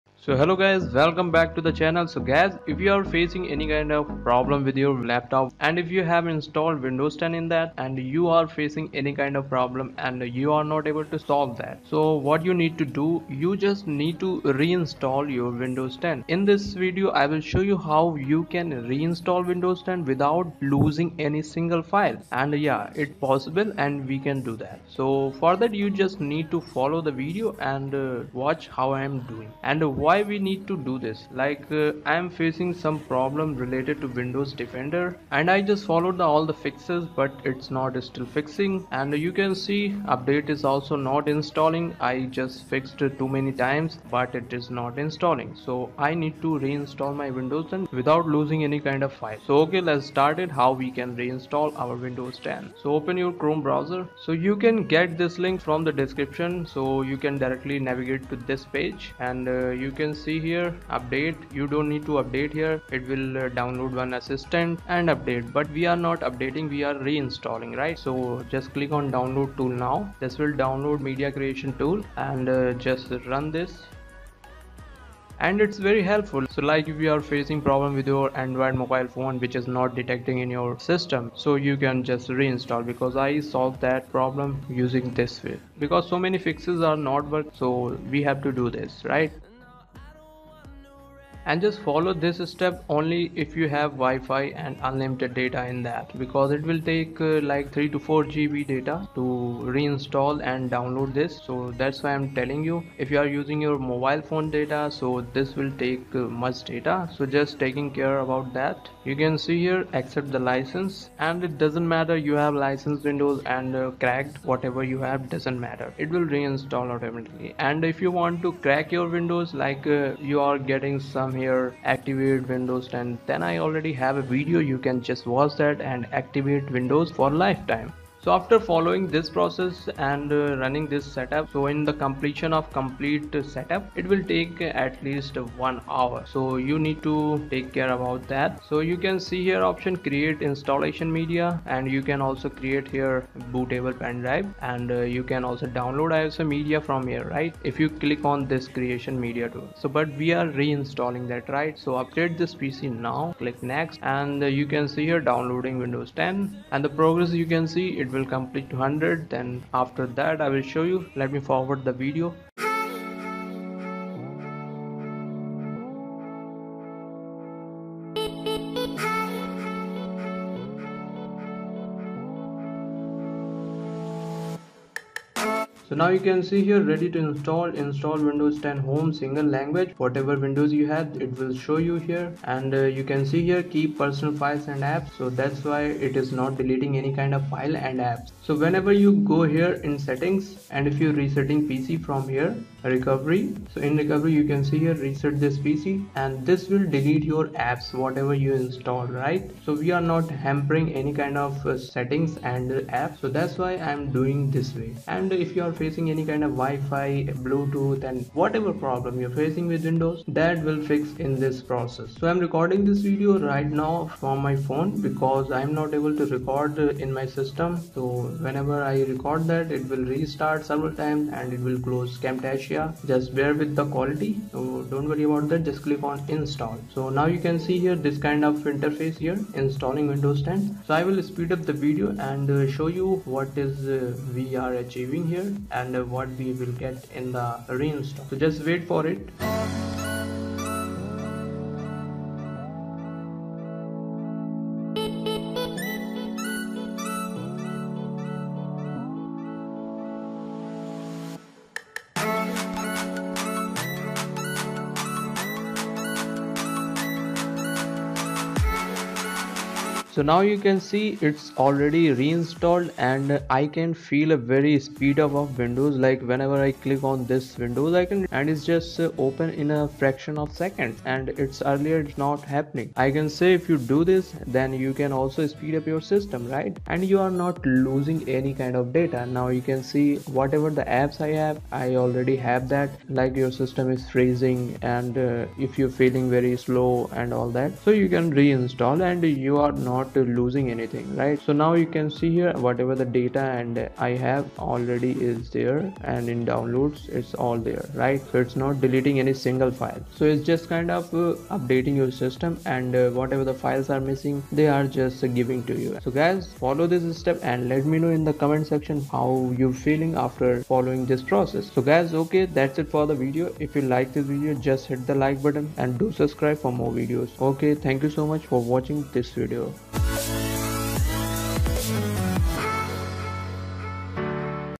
How to Reinstall Windows 10/11 Without Losing Data! | Reset Windows 10 (2024)
Hello, Buddy if you read this description thank you, In this video, I showed how we can Reinstall Windows 10 Without losing apps and data or files on any drive this will not delete anything from your system. So watch this video completely and see what steps this process going through.

If you have some troubles like :
👉Windows start menu not working.
👉Windows apps not working.
👉Corrupt system files.
👉Malware did damage to the operating system.
👉Failed Windows 10 updates and upgrades.
👉Windows Update that has corrupted the operating system.